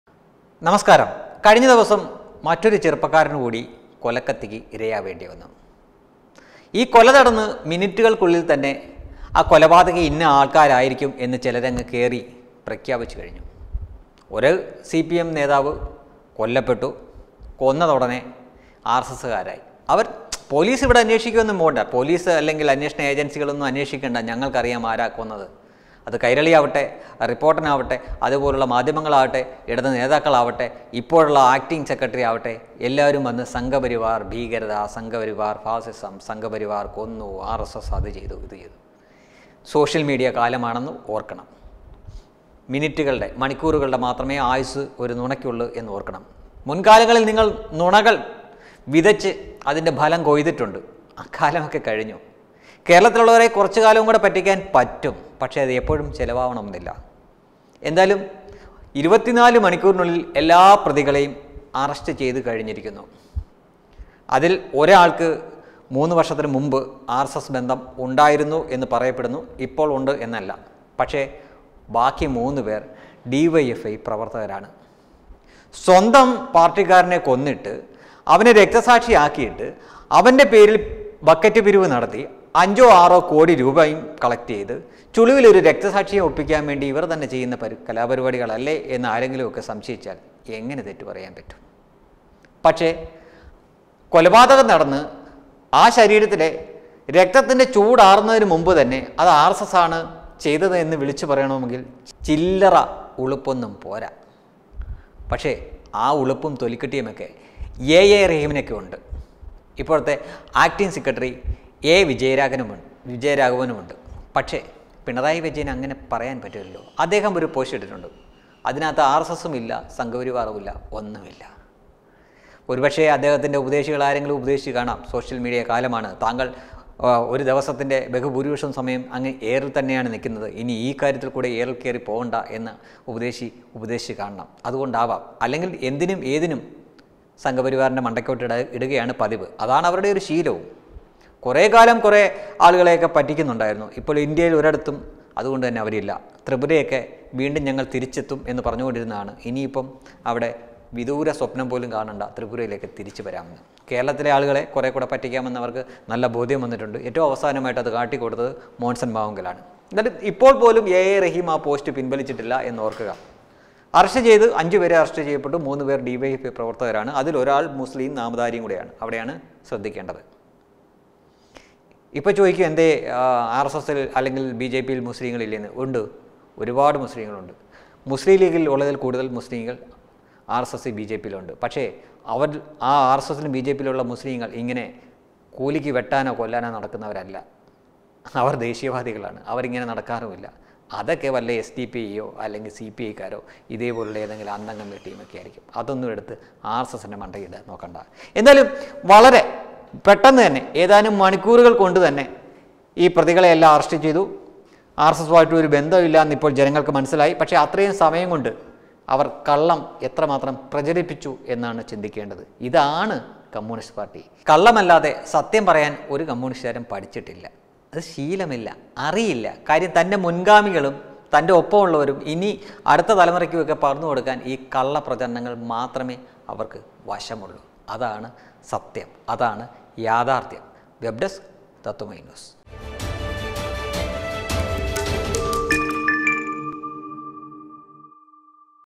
തിരുവല്ല കൊലപാതകത്തിൽ യെ യെ റഹീമിനെ തള്ളി പിണറായി വിജയൻ | THIRUVALLA MURDER
യെ യെ റഹിം പിന്നെയും പിന്നെയും എയറിൽ തന്നെ | A A RAHIM